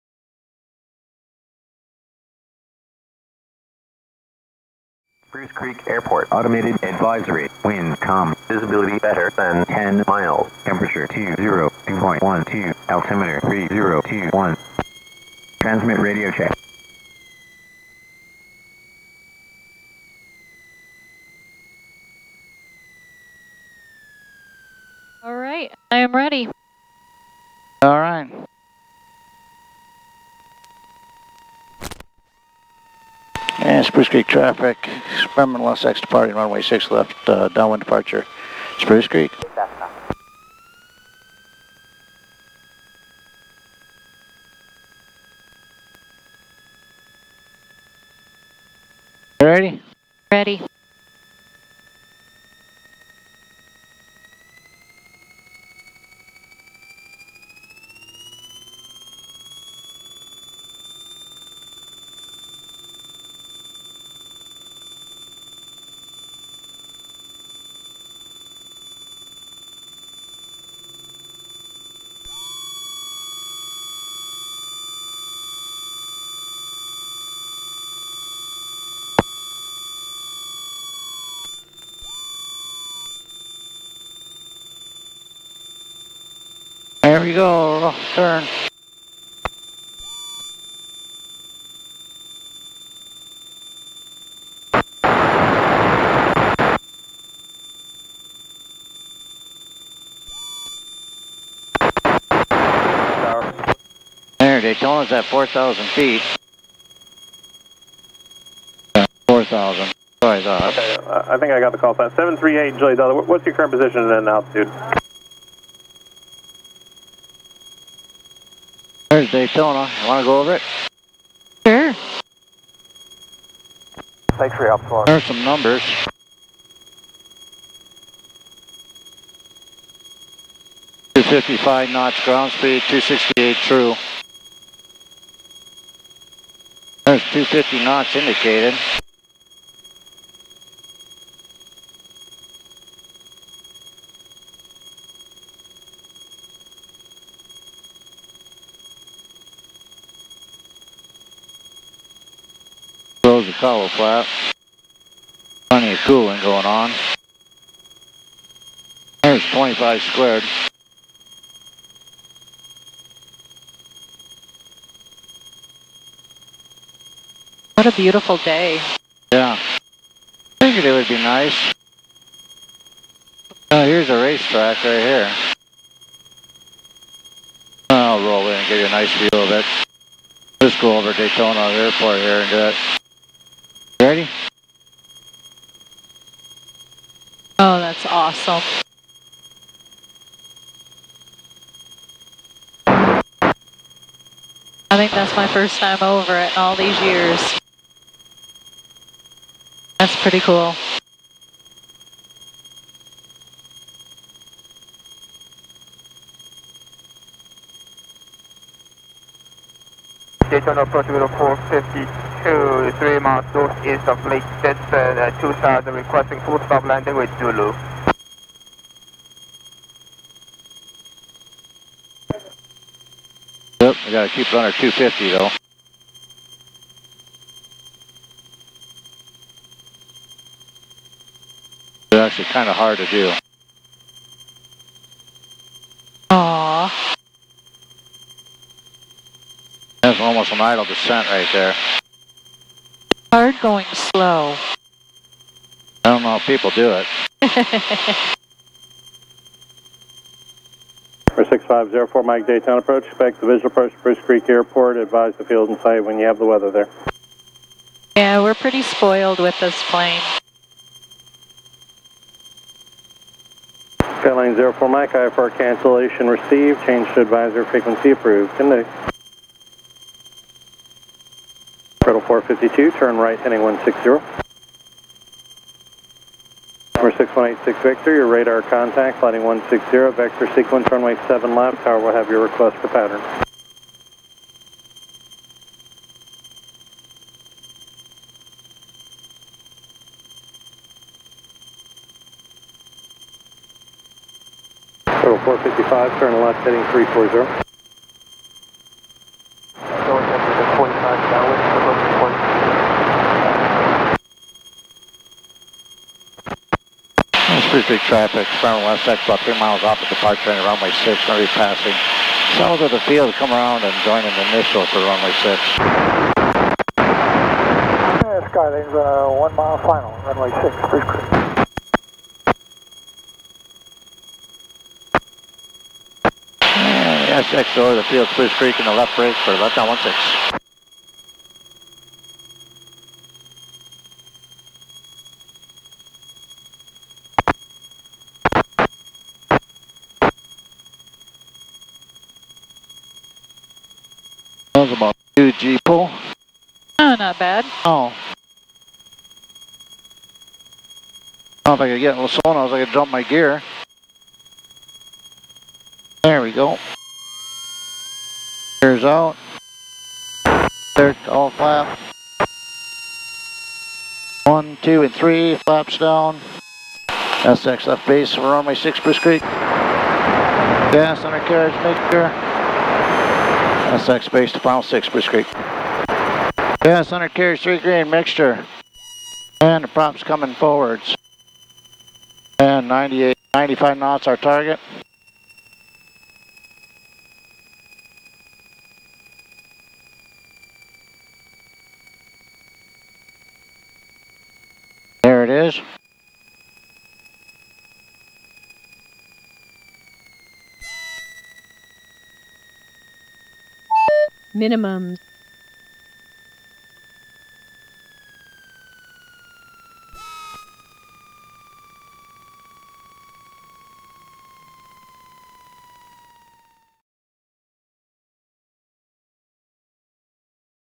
FLYING OVER DAYTONA INTL SPEEDWAY & THE FLORIDA COASTLINE | SX300 AIRPLANE
We finally have audio after 2 failed attempts! The media mod case around my GoPro was not allowing the audio cable to connect. We decided to try it out without the case and it worked!